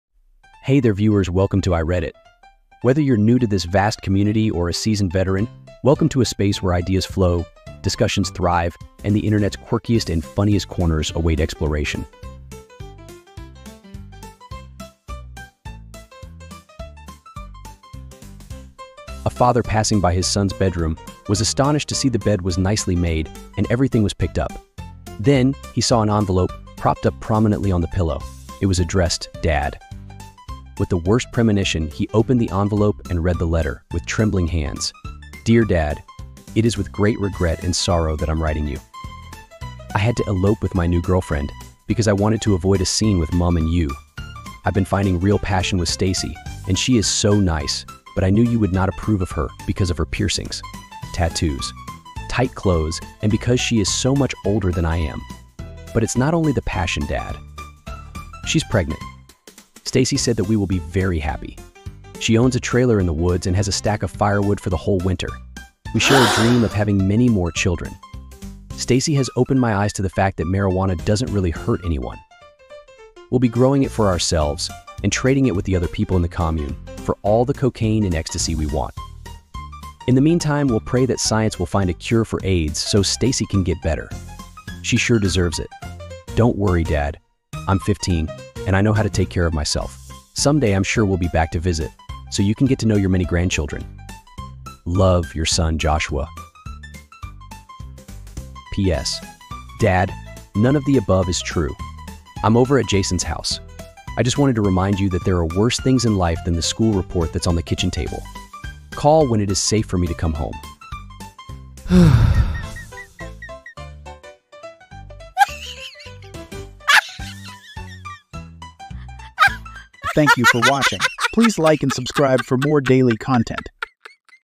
😂 Saturday's' Best Joke 😂 - Young Love, At Least He Left A Note
We will be sending content your way everyday and we hope to build a safe community which provides laughter, counsel and comfort!

                                                              👇Joke 👇

A father passing by his son's bedroom, was astonished to see the bed was nicely made, and everything was picked up. Then, he saw an envelope, propped up prominently on the pillow. It was addressed, "Dad." With the worst premonition, he opened the envelope and read the letter, with trembling hands... "Dear, Dad. It is with great regret and sorrow that I'm writing you.

I had to elope with my new girlfriend, because I wanted to avoid a scene with Mum and you. I've been finding real passion with Stacy, and she is so nice, but I knew you would not approve of her because of her piercings, tattoos, tight motorcycle clothes, and because she is so much older than I am. But it's not only the passion, Dad.

She's pregnant. Stacy said that we will be very happy. She owns a trailer in the woods, and has a stack of firewood for the whole winter. We share a dream of having many more children. Stacy has opened my eyes to the fact that marijuana doesn't really hurt anyone. We'll be growing it for ourselves, and trading it with the other people in the commune, for all the cocaine and ecstasy we want.

In the meantime, we'll pray that science will find a cure for AIDS, so Stacy can get better. She sure deserves it! Don't worry, Dad. I'm 15, and I know how to take care of myself. Someday, I'm sure we'll be back to visit, so you can get to know your many grandchildren. Love, your son, Joshua. P.S. Dad, none of the above is true.

I'm over at Jason's house. I just wanted to remind you that there are worse things in life than the school report that's on the kitchen table. Call when it is safe for me to come home!